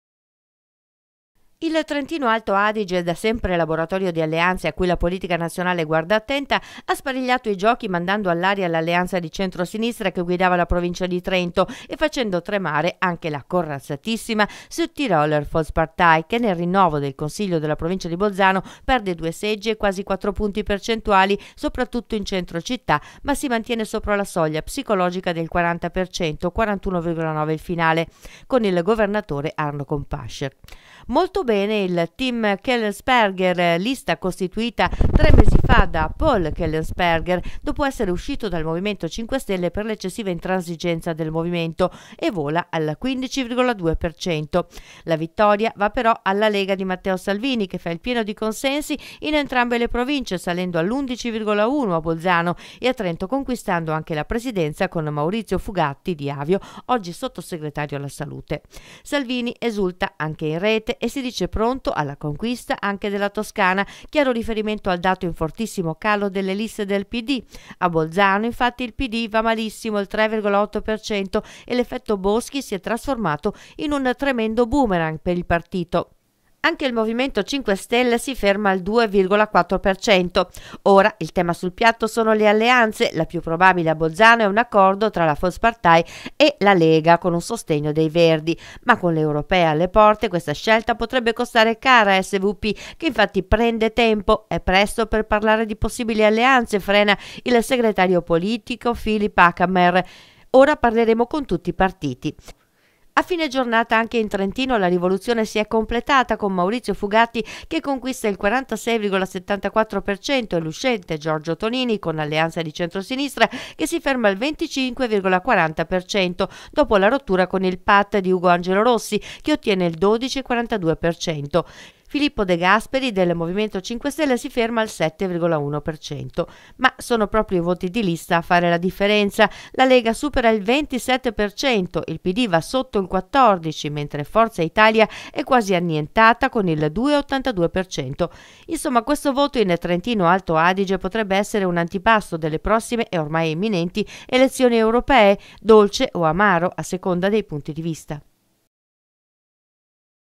SI CAMBIA
Le elezioni in Trentino Alto Adige premiano la Lega, e Matteo Salvini esulta sul web. Ma ora si apre un tema di alleanze a Bolzano, dove SVP mantiene comunque un buon quasi 42% dei consensi. Fugatti conquista Trento, ma dovrà lasciare la carica di sottosegretario alla salute.